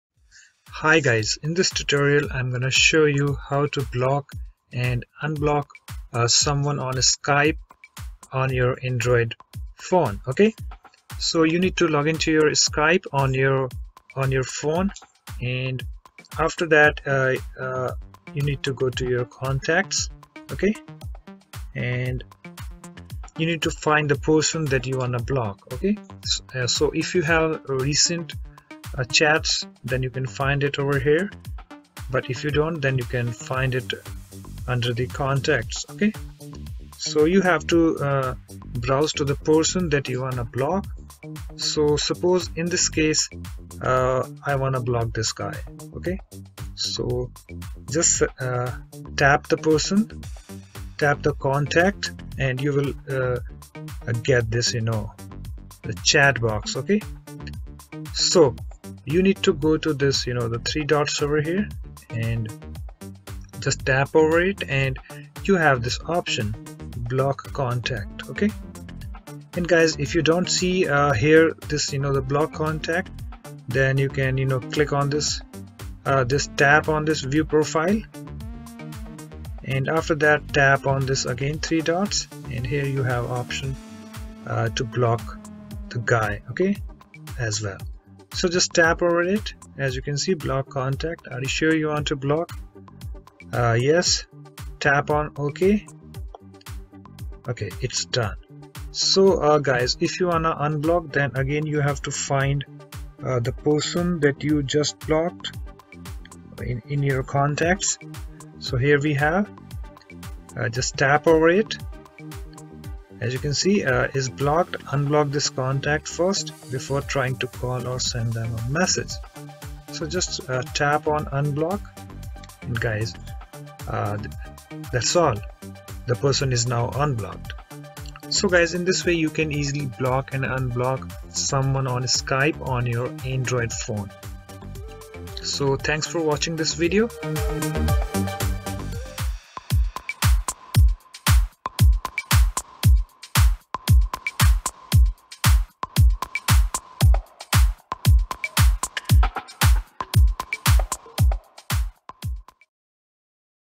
How to Block and Unblock Someone On Skype Android App -2016 ?
How to Block and Unblock Someone On Skype Android App ?

To Block :

Step 1: Open Skype App On Your Phone .

Step 2: Go to Contacts and Browse to the Person that you want to block .Tap On it .

Step 3: Now ,Tap On three dots and Tap on Block Contact .

To Unblock :

Step 1: Go to Contacts and Browse to the person that you want to unblock ,then tap on it .

Step 2: Select Unblock .

That's all